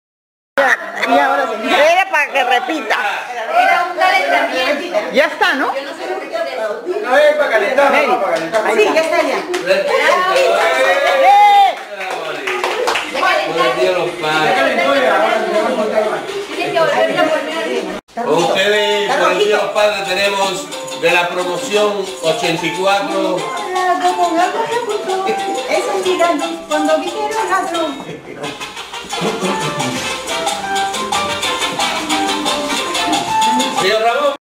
BAILANDO DESPUES DEL FIESTON
DE AMANECIDA Y CASI SIN DORMIR LA ALEGRIA CONTINUABA